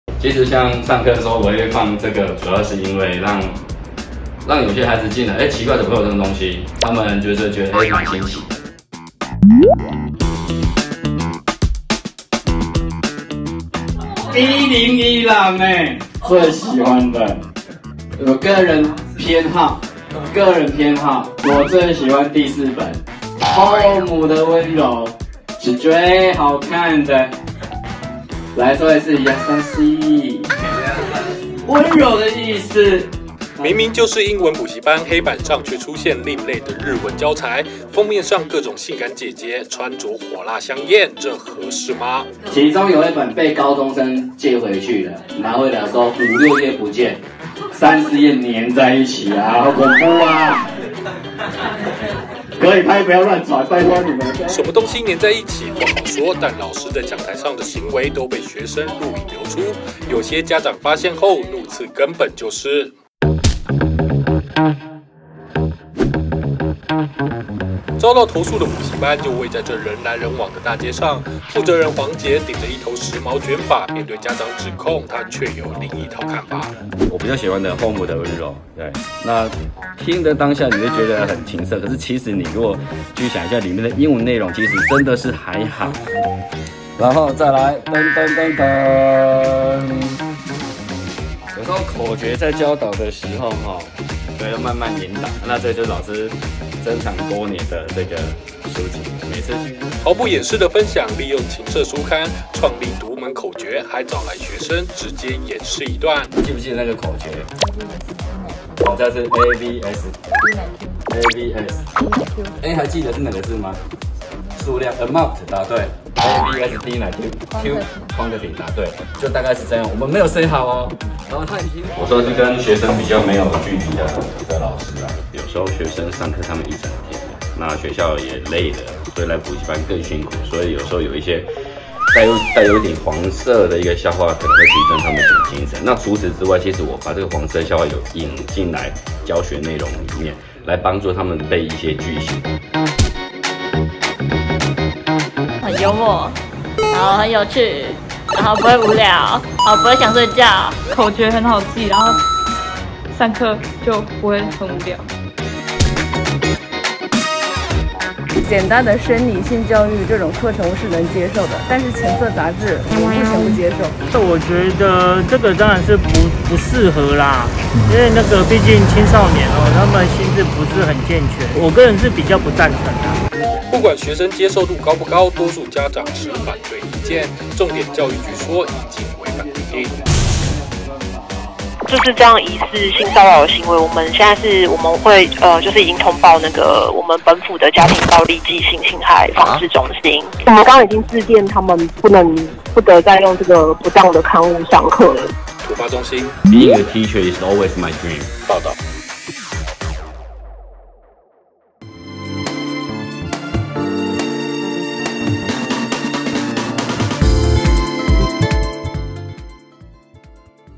獨家｜「後母的溫柔」情色書刊當教材 補教師：有助記憶終生受用 | 台灣新聞 Taiwan 蘋果新聞網
蝦毀！18禁情色雜誌也能當英語教材？桃園一間私立國高中英語補習班， 驚見英文老師教學時拿出多本「18禁」情色雜誌當作補充教材，利用雜誌封面穿著清涼火辣女子圖片，吸引台下聽課學生注意力。過程中學生雖笑到「美叮每噹」，也有學生拿出手機拍下導師教學過程，並錄製成「抖音」公開，但這樣相當另類的教學方法也引起家長關注，認為十分不妥。補習班英文老師黃杰則解釋，帶點黃色笑話提升學生注意力，並融入英語句型教學，有助學生記憶終生受用；學生則是和家長看法兩極，覺得這樣確實較容易記憶；桃園市教育局則表示，教學恐涉及性騷擾，已轉相關單位調查。

【獨家｜「後母的溫柔」情色書刊當教材　補教師：有助記憶終生受用】